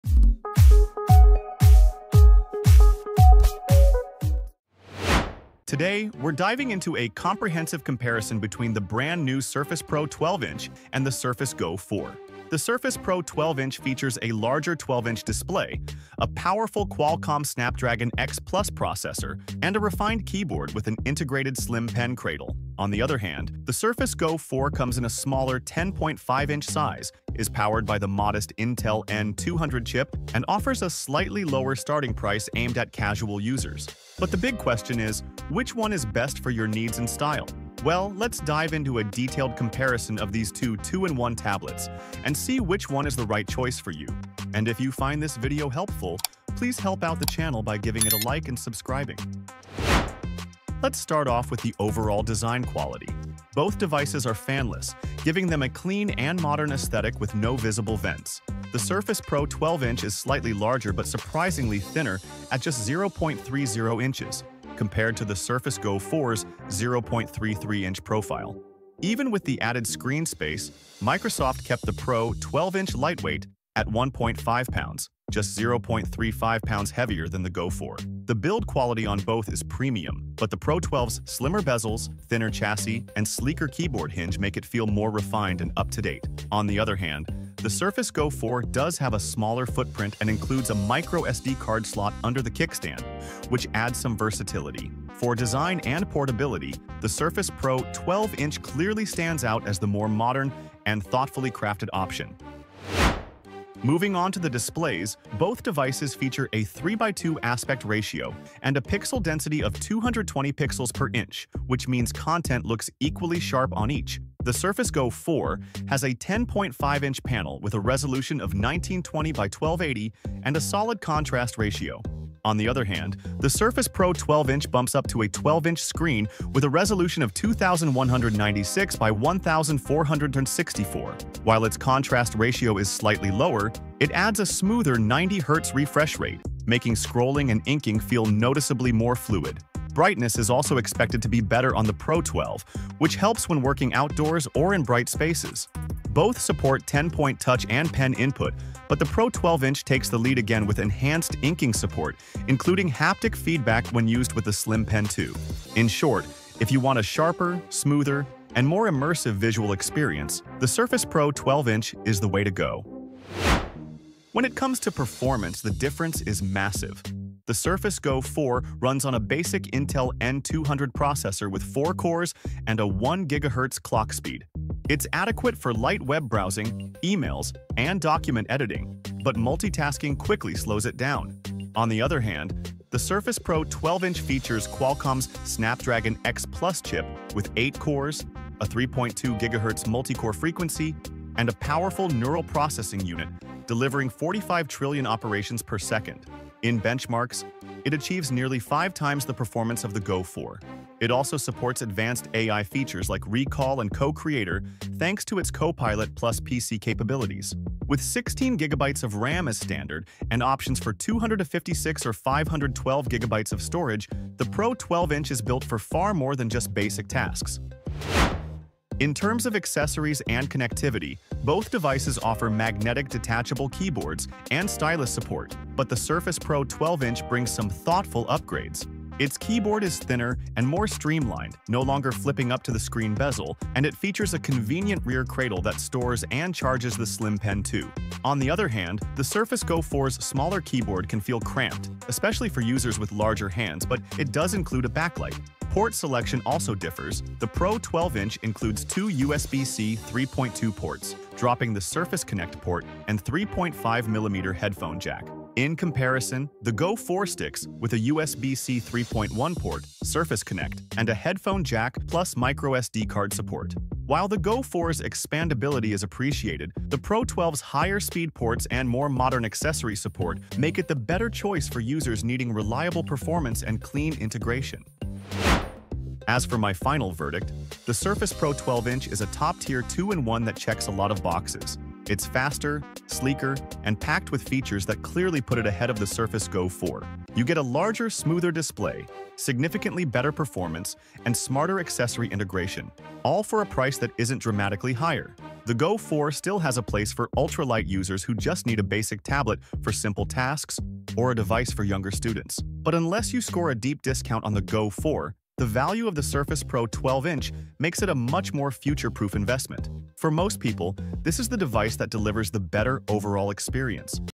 Surface Pro 12 vs Surface Go 4 – Which Windows Tablet Should You Buy in 2025?
Surface Pro 12 vs Surface Go 4 – Full Comparison for 2025 Buyers

Surface Pro 12" (Snapdragon X Plus)
Surface Go 4

Thinking about upgrading your Windows tablet? In this video, we dive into a detailed comparison between the newly launched Surface Pro 12-inch and the compact but capable Surface Go 4. We’ll cover everything from display quality and performance to battery life, portability, and who each device is best suited for. Whether you’re a student, creative, or on-the-go professional, this video will help you pick the right device in 2025.

⚡ Ideal for digital nomads, students, and multitaskers who need a hybrid Windows device with performance and portability.

CHAPTERS
00:00 Intro
00:49 Design & Build
01:43 Display Features
02:43 Performance Power
03:38 Accessories & Ports
04:35 Verdict